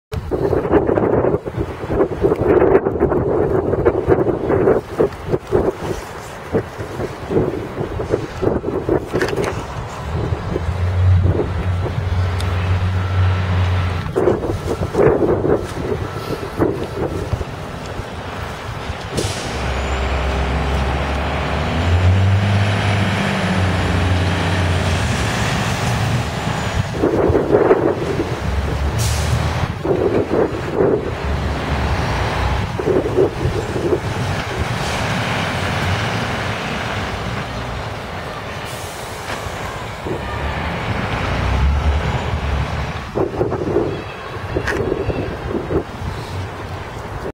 Rescue pump & Pod Truck Strathclyde Fire & Rescue
A Scania rescue pump and a MAN pod truck is seen here returning to Renfrew fire station form a call in the city